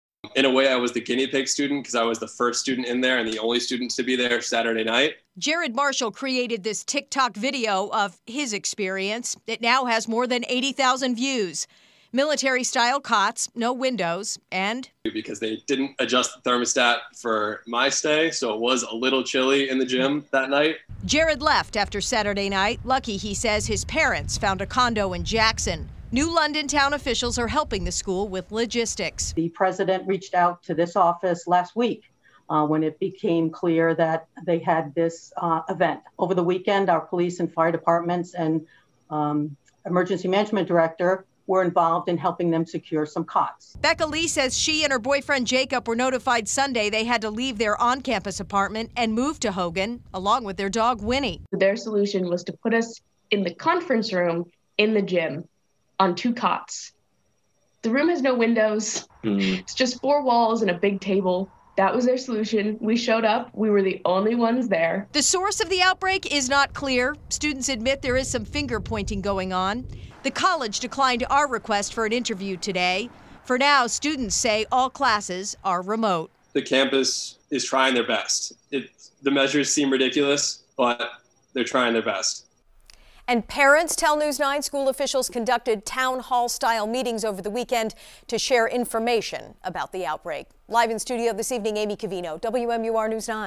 Colby-Sawyer sets up surge housing in gym after COVID-19 cases increase
Colby-Sawyer College in New London is dealing with a surge in COVID-19 cases, with 57 positive cases in a population of 855 students.